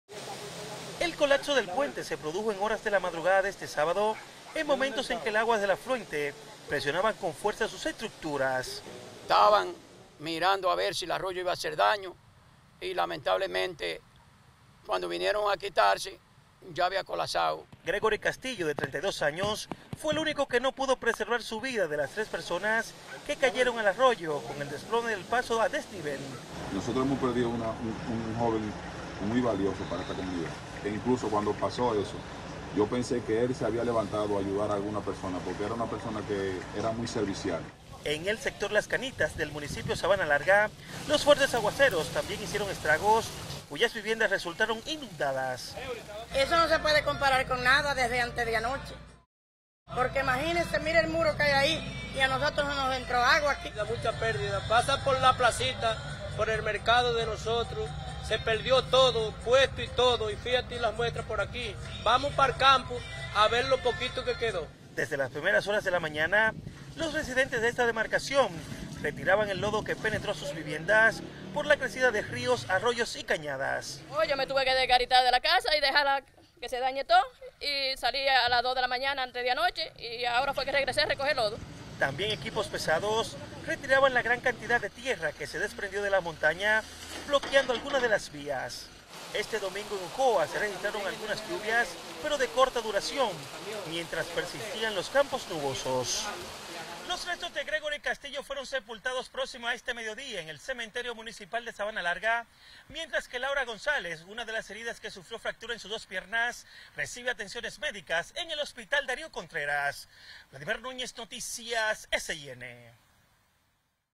Ocoa: un muerto y 2 heridos tras desplome puente
Un muerto y dos personas heridas fue resultado del desplome de un puente sobre el arroyo Las Vacas de la comunidad El Limonal de Ocoa fruto de las intensas lluvias registradas por disturbio tropical, cuyos efectos se sintieron con mayor fuerza este sábado dejando serias inundaciones en decenas de viviendas y bloqueo en algunas de las carreteras de la provincia sureña.